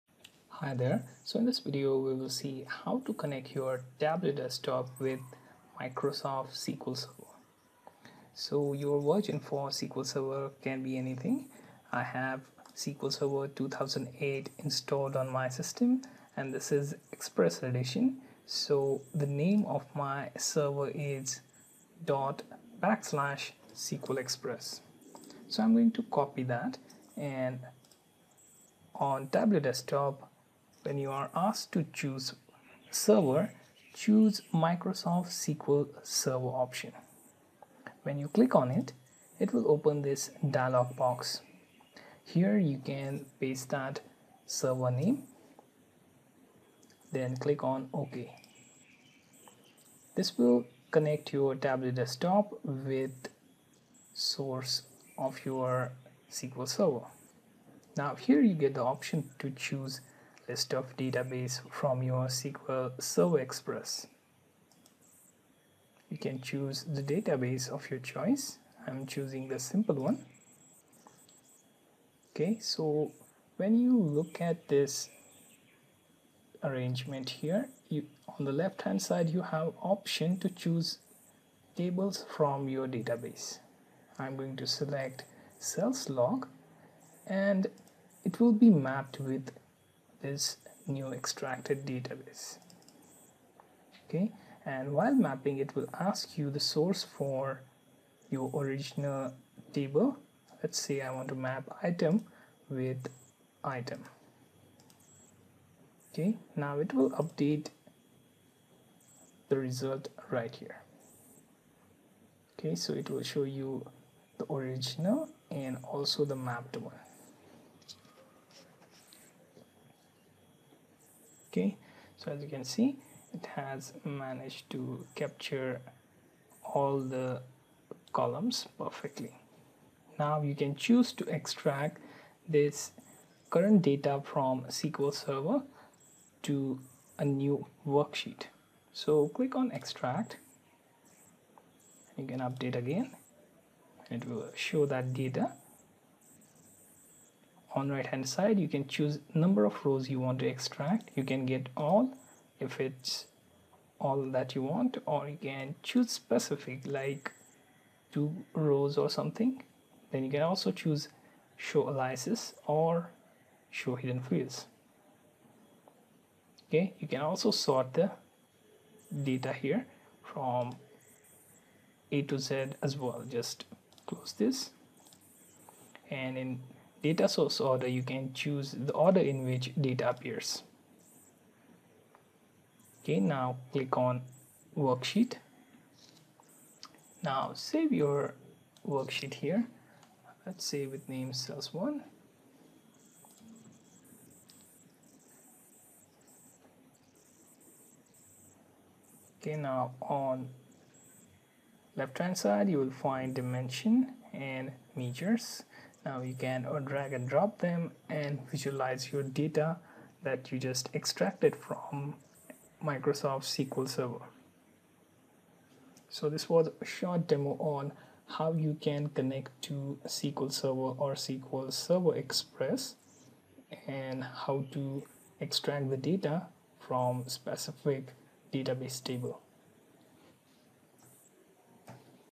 Tableau Desktop Connect to SQL Server
Learn how to Connect to SQL Server in Tableau desktop.